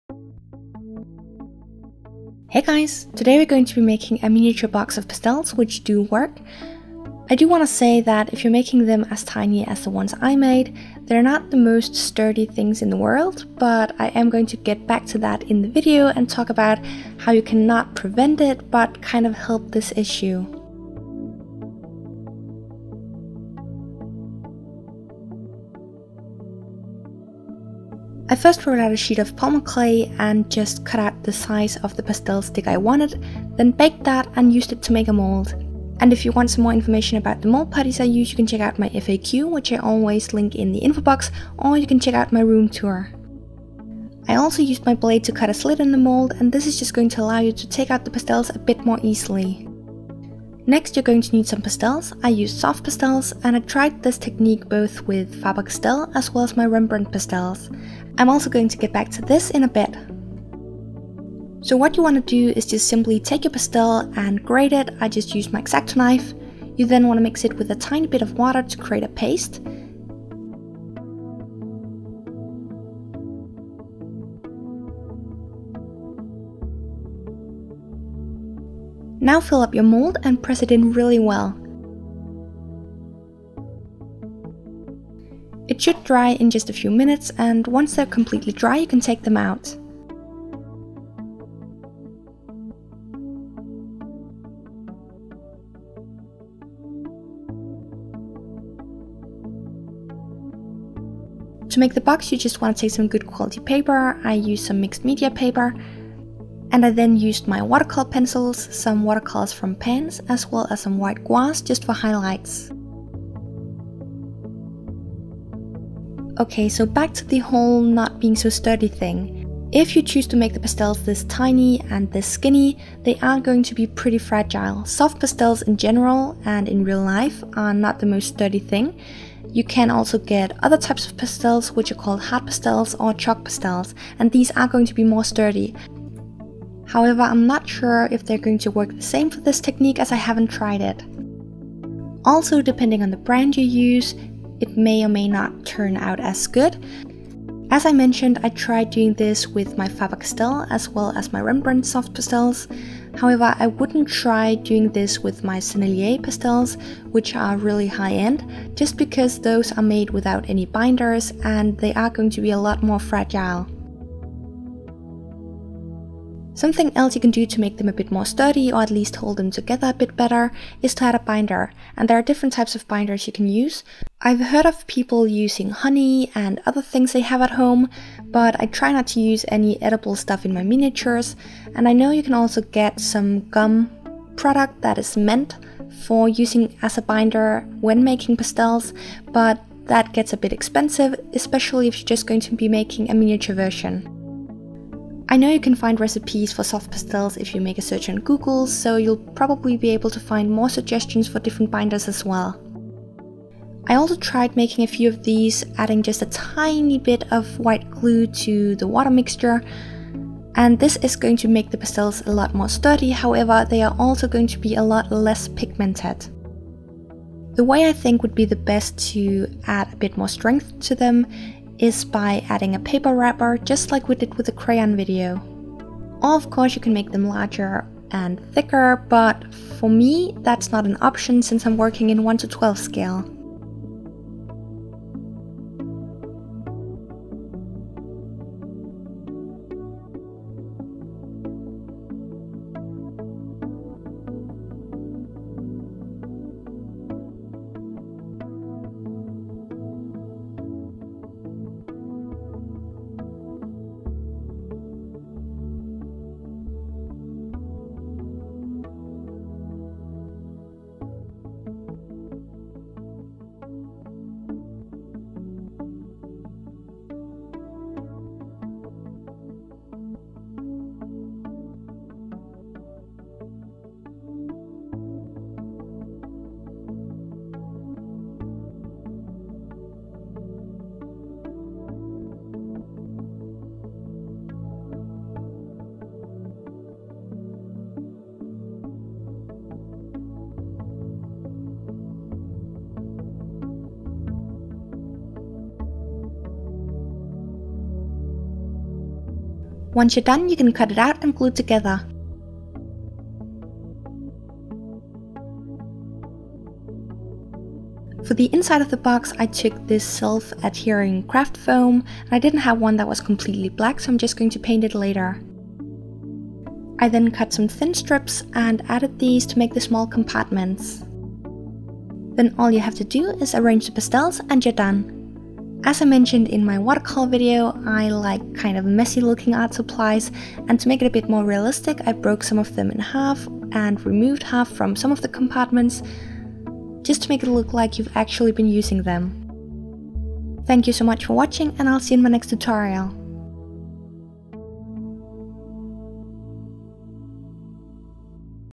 Tiny Art Supply Tutorial ; Box Of Pastels (That work)
Hey guys! Today's video is not a polymer clay tutorial - in fact, I only used polymer clay for the very first step :)
Today we're making a miniature box of pastels, or soft pastels - and If you're interested, I also have a tutorial showing how to make the watercolor palette as well as some crayola inspired cayons.

These are 1:12th scale for dollhouses, but they'll be perfect to use with Barbie, Monster high, LPS, or even American girl if size is made accordingly :)